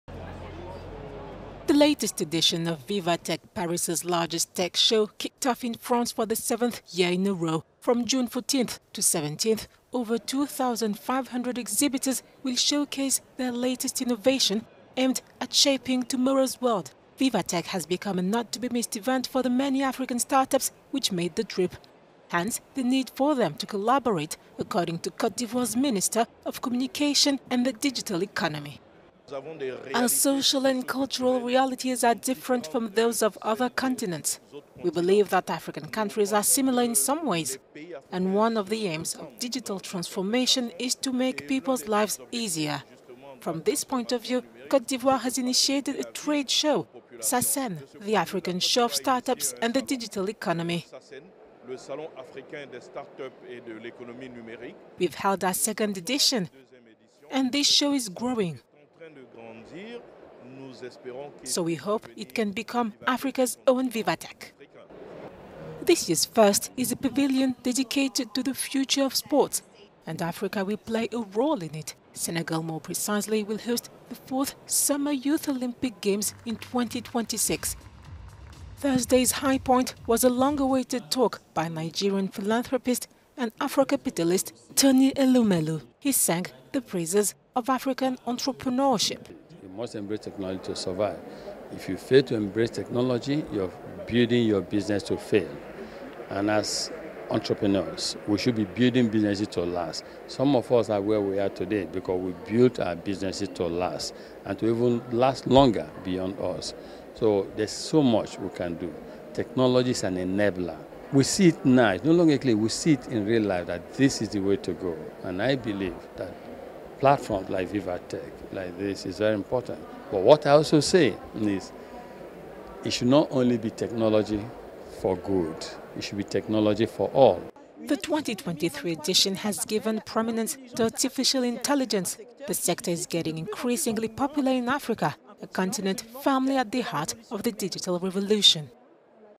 VivaTech forum: Africa at the heart of the digital revolution
The 2023 edition has given prominence to artificial intelligence. The sector is getting increasingly popular in Africa, a continent firmly at the heart of the digital revolution. From June 14th to 17th, over 2,500 exhibitors will showcase their latest innovation.